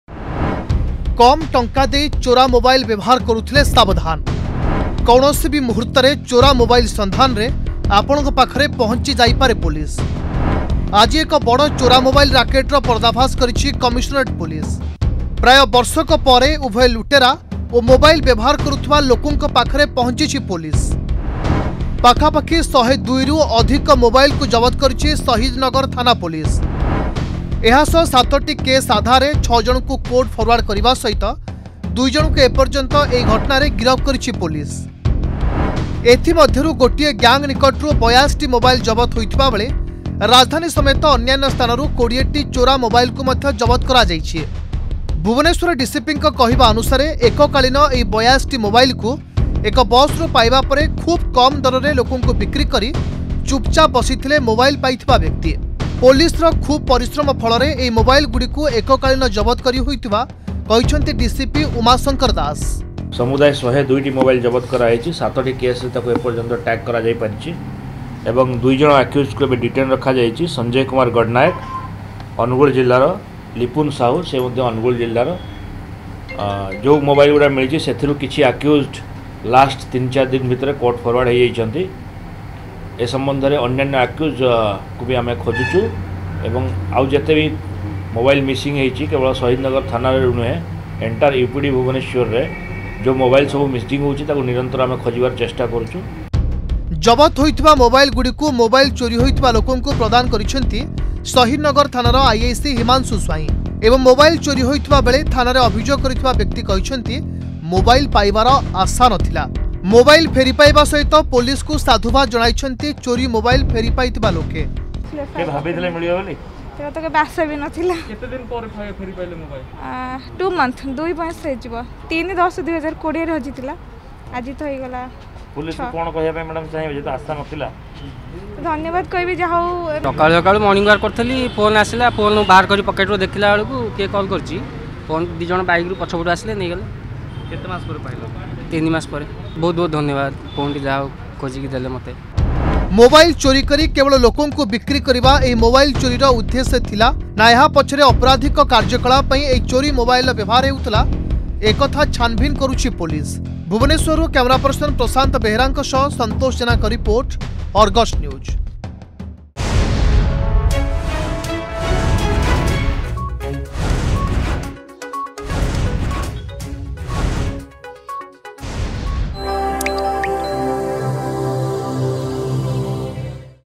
ରାସ୍ତାରେ ପଡିଥିବା ମୋବାଇଲ ଉଠାନ୍ତୁ ନାହିଁ, ନଚେତ ଧରିପାରେ ପୋଲିସ
କମ ଟଙ୍କା ଦେଇ ଚୋରା ମୋବାଇଲ ବ୍ୟବହାର କରୁଥିଲେ ସାବଧାନ । କୌଣସି ବି ମୁହୁର୍ତ୍ତରେ ଚୋରା ମୋବାଇଲର ସନ୍ଧାନରେ ଆପଣଙ୍କ ପାଖରେ ପହଁଚି ଯାଇପାରେ ପୋଲିସ । ଆଜି ଏକ ବଡ ଚୋରା ମୋବାଇଲ ରାକେଟର ପର୍ଦ୍ଦାଫାସ କରିଛି କମିଶନରେଟ ପୋଲିସ...